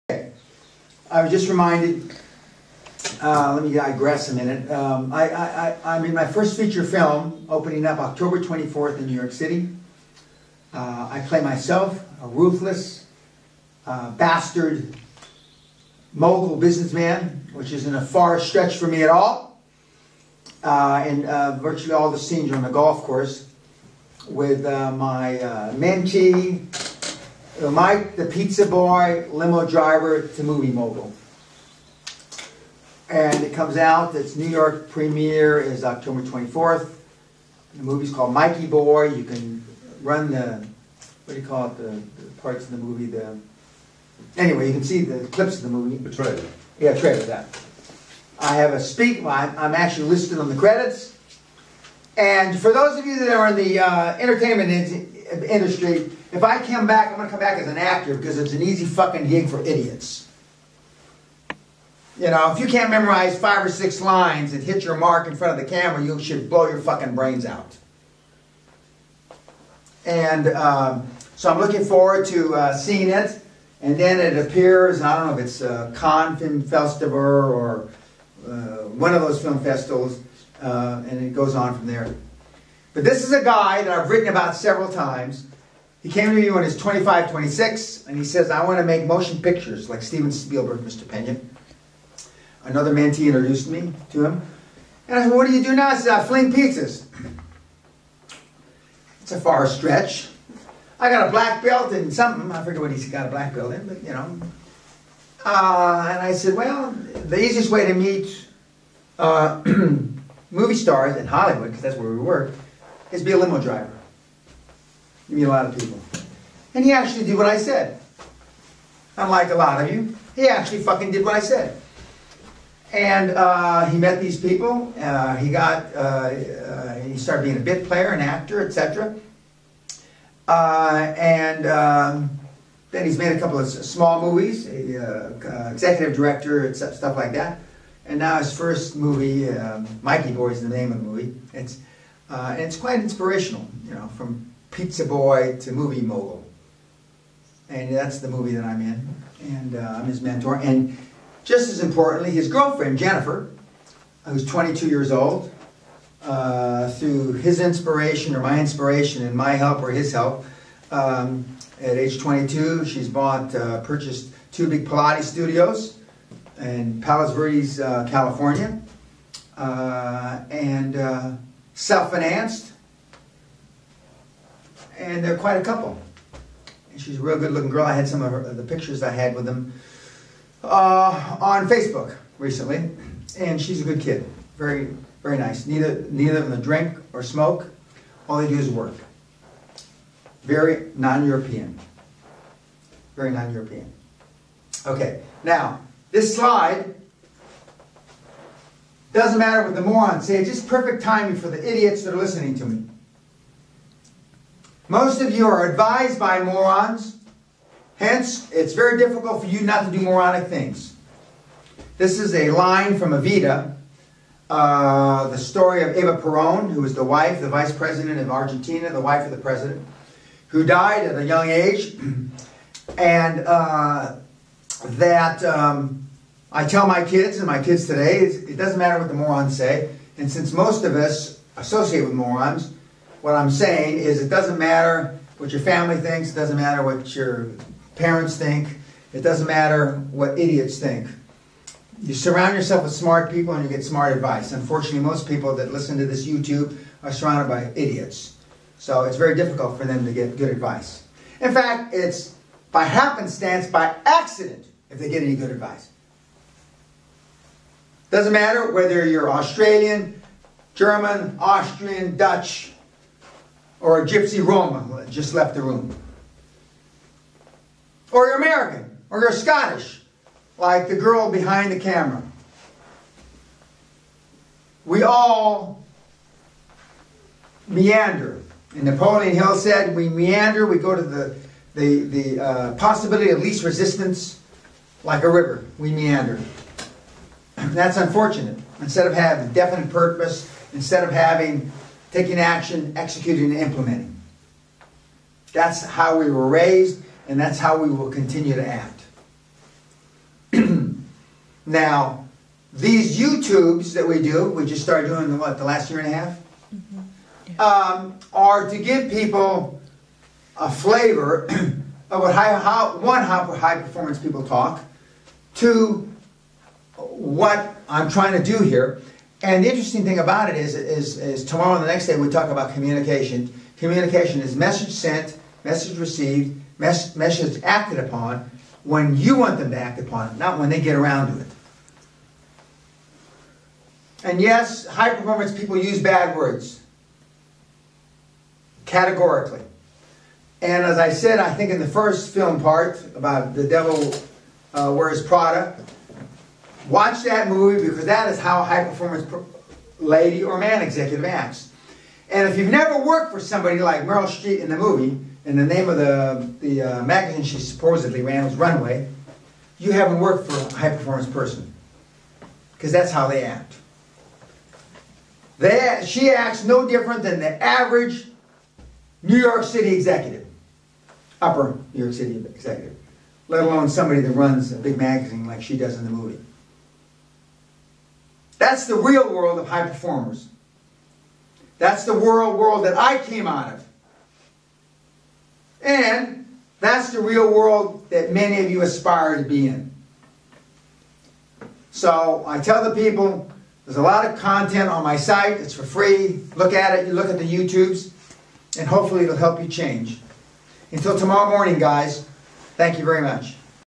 Day 1 Part 3 | September 2013 | Dan Peña's QLA Seminar
Dan Pena September 2013 QLA Seminar at Guthrie Castle with attendees in the Pavilion Seminar Room. You can download Dan's newly released "$50 Billion QLA Ebook" for free

TAKE THE FREE SUCCESS TEST NOW!